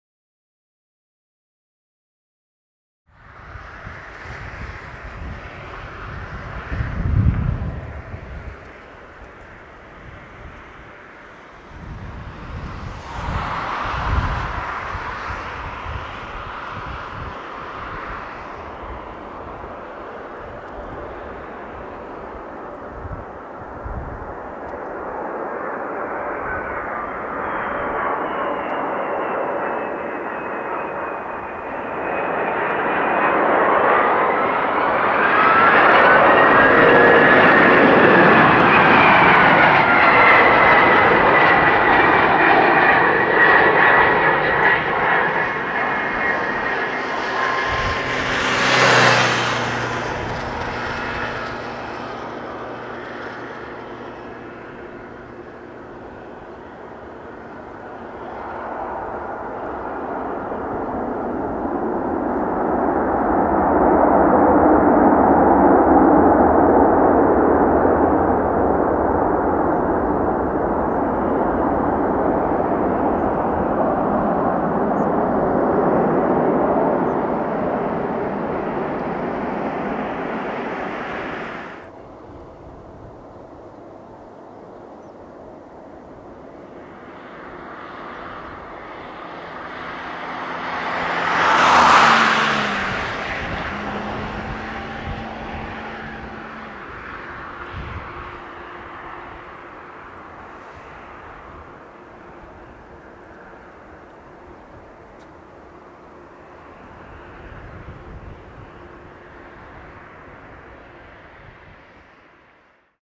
[747 classic] Kalitta Air Boeing 747-200F (N794CK) landing at NRT/RJAA (Tokyo - Narita) RWY 34L
Kalitta Air Boeing 747-222B(SF) (N794CK) landing at NRT/RJAA (Tokyo - Narita Int'l Airport in Japan) on runway RWY 34L.
Date: Feb. 28, 2015 6:12-6:14 (UTC+9)
Runway: 34L
Location: 34L end
Coordinates for Google Map: N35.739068, E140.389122
Flight: K49734 from ATL via ANC
Camera: SONY HDR-CX430V